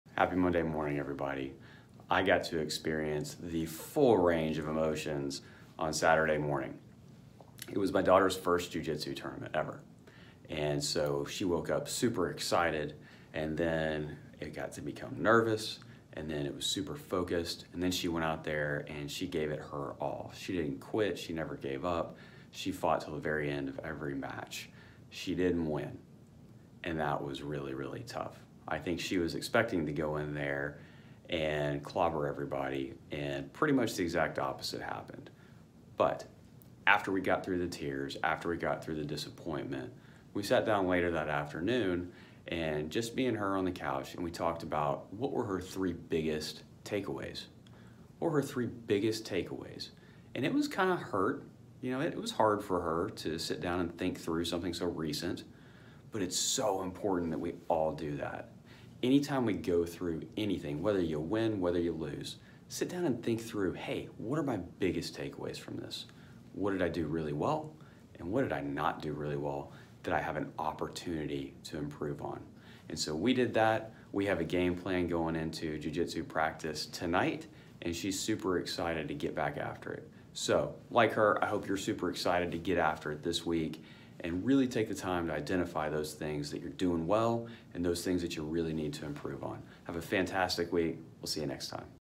Motivational Monday | Ep. 34
I experienced a full range of emotions on Saturday as my daughter had her first jiu-jitsu tournament. She went into it thinking she was going to win every fight, but things didn't go as planned.

While she was very disappointed and there were tears shed, we worked through it together. We sat down and talked about her 3 biggest takeaways from the tournament and have created a game plan for jiu-jitsu practice tonight.

This is a lesson I have learned through years of practicing law and just living life. Make sure you are learning from your experiences, whether you win or lose!